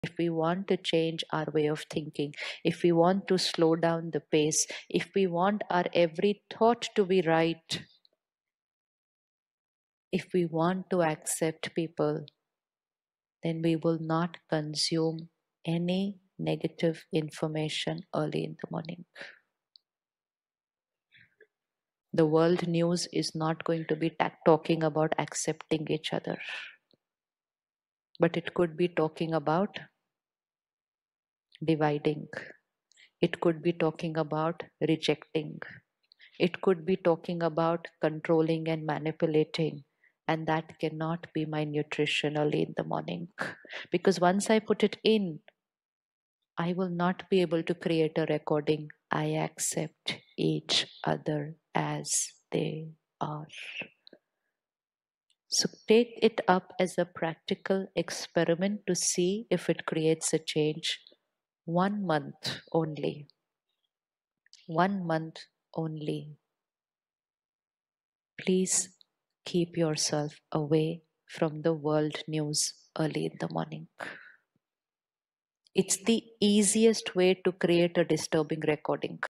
Transform Your Mindset: The Morning Experiment with BK Shivani 🌅✨
BK Shivani emphasizes the importance of starting your day without negative information. If we want to change our way of thinking, slow down our pace, and embrace acceptance, we must avoid consuming negativity first thing in the morning. World news often focuses on division, rejection, and manipulation, which is not the nourishment we need for a positive mindset. Instead, commit to a one-month experiment: avoid world news in the morning and notice how it transforms your thoughts and recordings of acceptance.

🙏 Support Silent Strength: Help create a charity focused on supporting individuals with social anxiety. Every contribution makes a difference Thank you for your support!